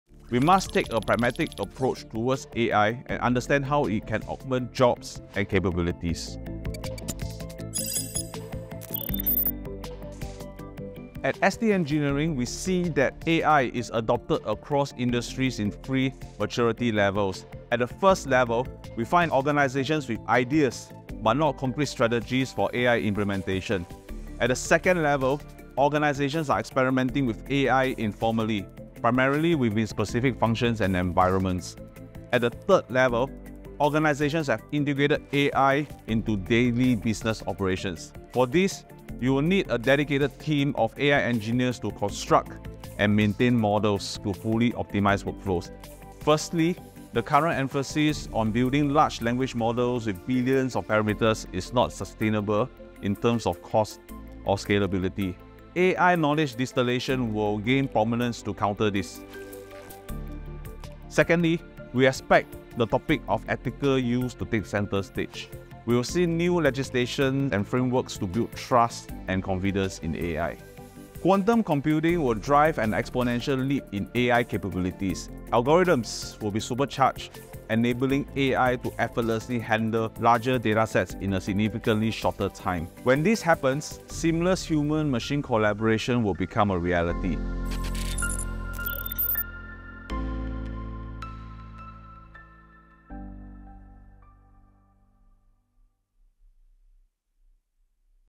Working Alongside Artificial Intelligence
Organisations with a pragmatic approach towards AI unlock smarter decisions and create more innovative products and services. With our scalable data science, analytics and AI solutions, we know what it takes to derive valuable and actionable insights.

Watch as Tan Boon Leong, Head of DeepBrain, Digital Systems gives his insights on staying ahead in the AI revolution.